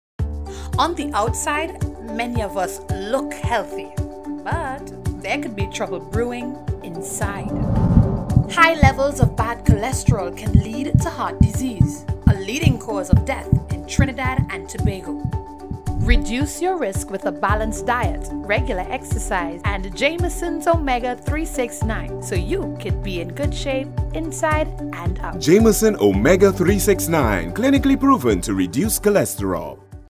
JAMIESON OMEGA 3-6-9
JAMIESON OMEGA 3-6-9 IS A COMPLETE SOURCE OF OMEGA-3, 6 AND 9 ESSENTIAL FATTY ACIDS. DERIVED FROM FRESH, PURE COLD-PRESSED OILS OF ORGANIC FLAX, BORAGE AND SUSTAINABLY SOURCED FISH. IT HELPS TO SUPPORT HEART HEALTH AND MAINTAINS HEALTHY BRAIN FUNCTION.